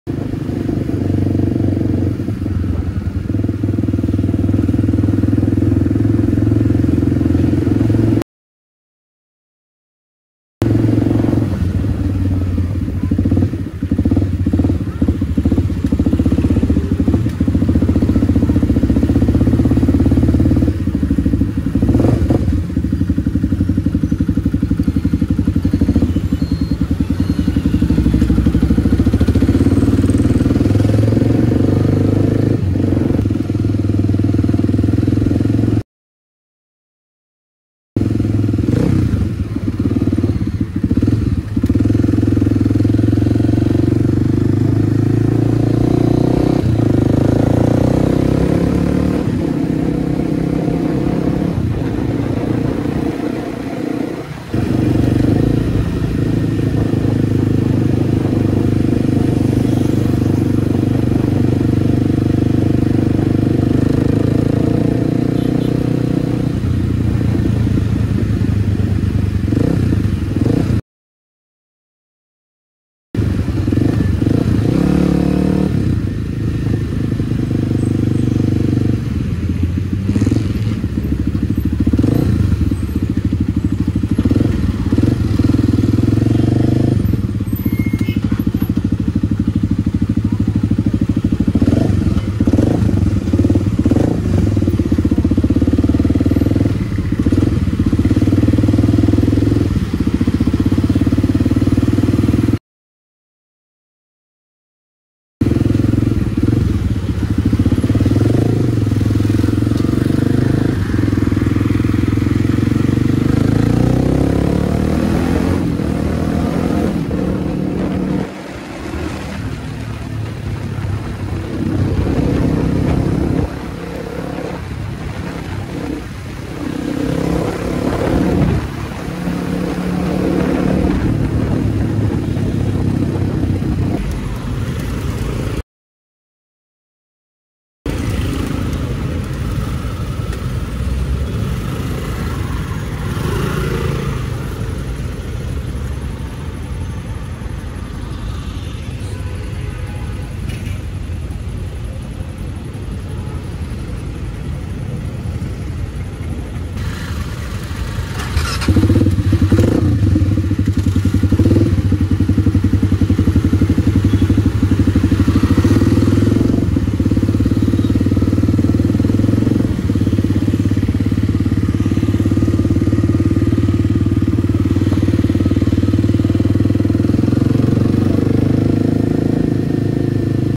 Daily Observations (Kawasaki D Tracker)
Daily Observations Moto Vlog
Featuring: Kawasaki D Tracker 250
Daily What Happening On The Road, Traffic Violation.
Pedestrian violations crossing behavior at signal intersections. No Helmets
Beautiful Nature
Peoples Reaction
Bad Road Condition